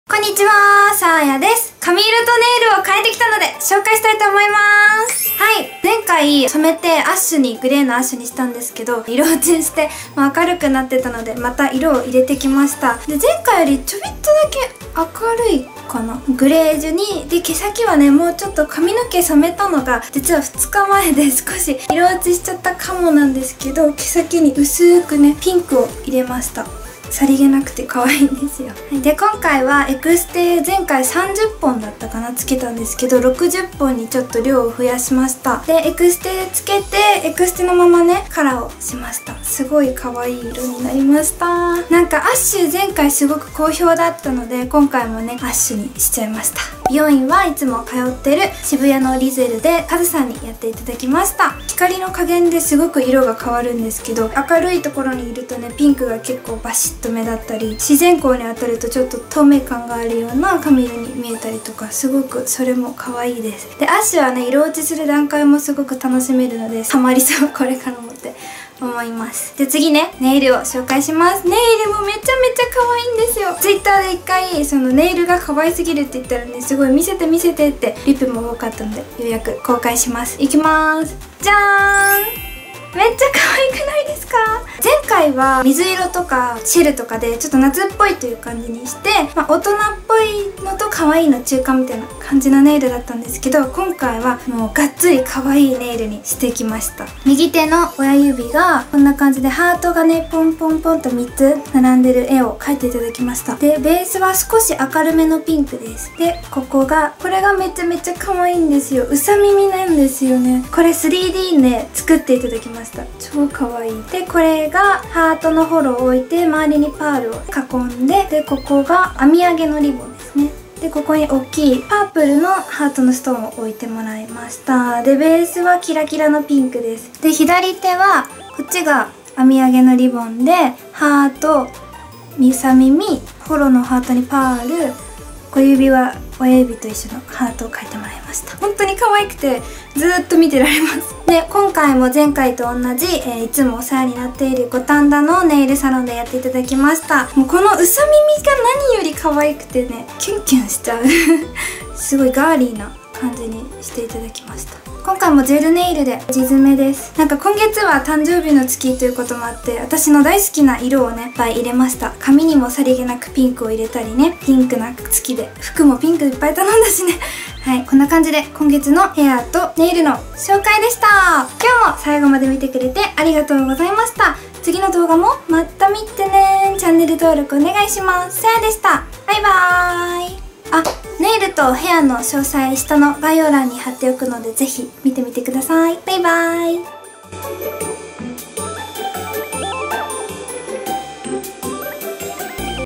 【NEW】髪色とネイル変えました！！！！！
こんにちは！さぁやです！！
新しい髪色とネイルの紹介です！

〜私の通ってる美容院〜
RISEL.xoxo.KAZU

〜私の通ってるネイルサロン〜
Beauty Nail Myu's
東京都品川区東五反田1-12-13　親切堂ビル4F

サブチャンネル（さやぴんく(さぁや2nd)）